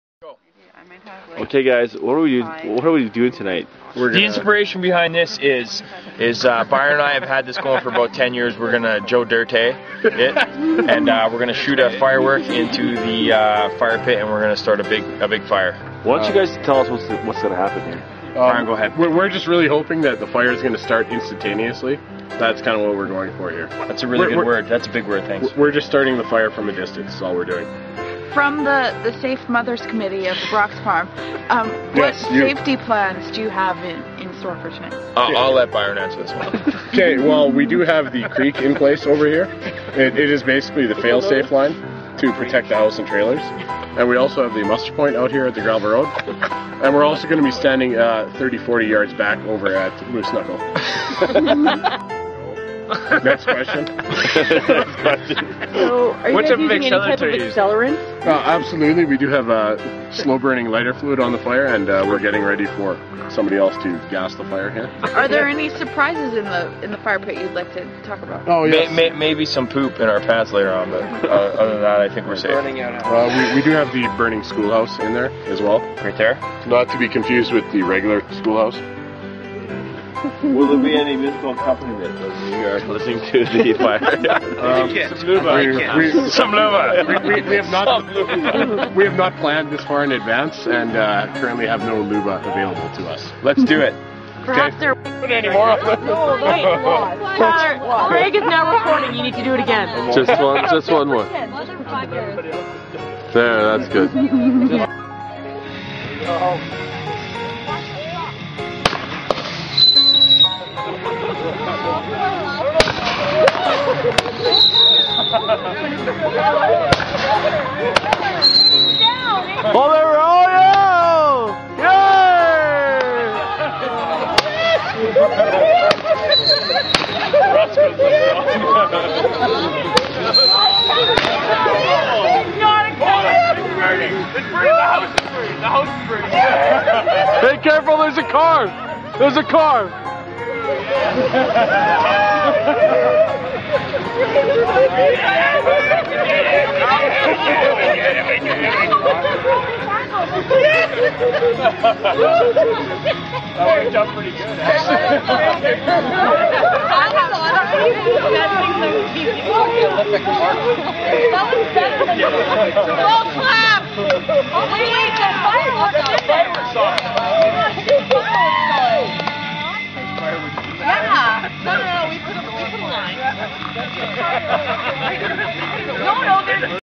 Roman Candle Lights Fire - Joe Dirt Style
Guys light fire with roman candles.